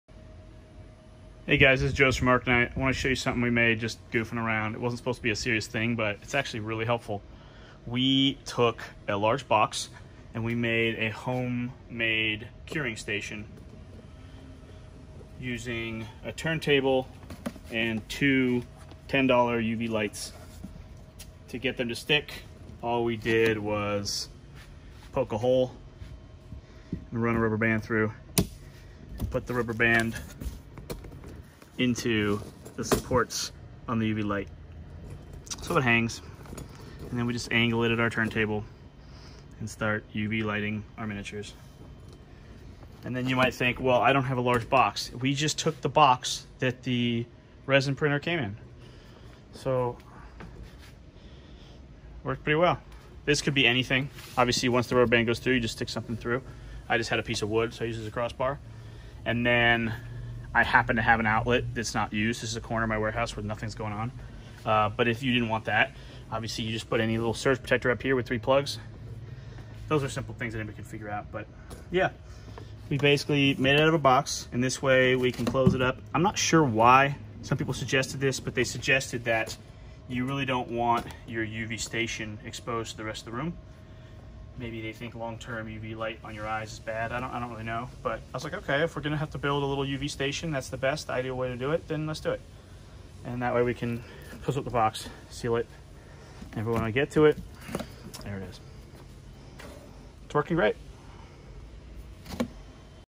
Home Made UV Curing Station for $20! 3D Resin Printing
Hey guys! Just a quick video showing off a UV curing station I built, using the box that my resin printer came in, and 2x $10 UV lamps I bought off Amazon.com.

It's working perfectly! I used a turntable that I already had, but this feature isn't required, you could just rearrange your minis while they cure.